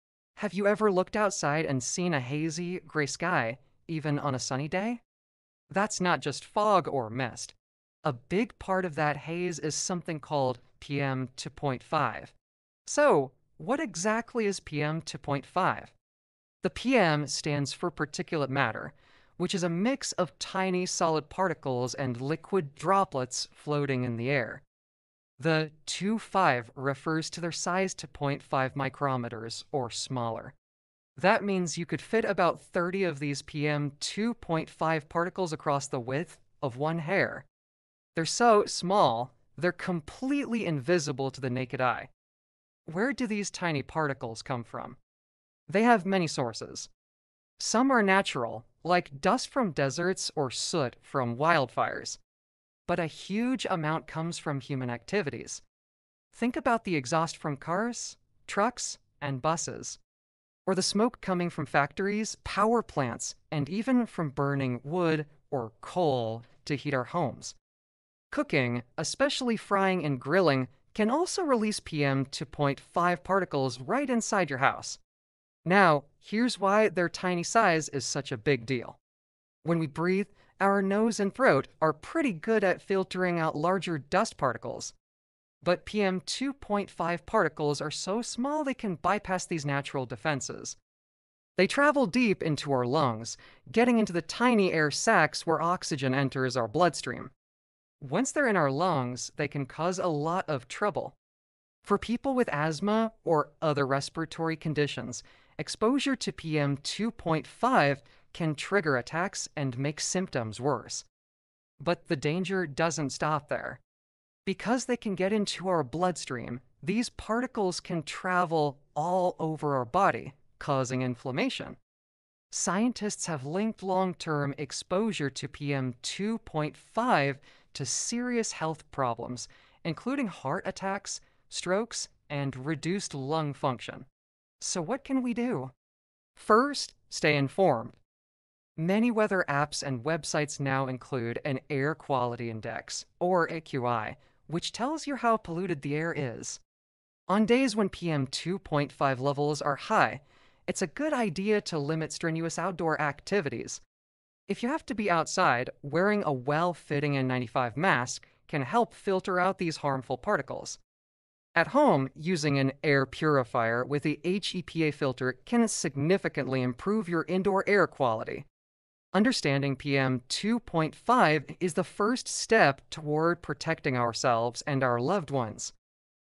PM2.5: The Invisible Dangerous Pollutant That Enters Your Lungs
Ever wonder why the sky looks hazy even when it’s sunny? The answer is PM2.5—tiny airborne particles so small they can slip deep into your lungs and even enter your bloodstream.
In this video, we break down what PM2.5 is, where it comes from, how it affects your health, and what you can do to protect yourself.
Stay informed. Stay safe.